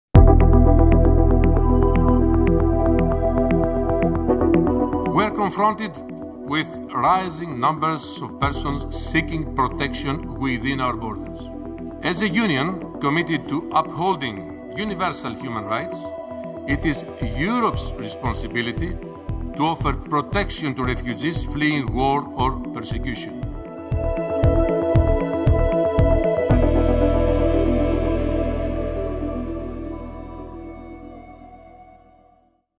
Dimitris Avramopoulos: Migration, Home Affairs and Citizenship
Meet Dimitris Avramopoulos, the new Commissioner for Migration, Home Affairs and Citizenship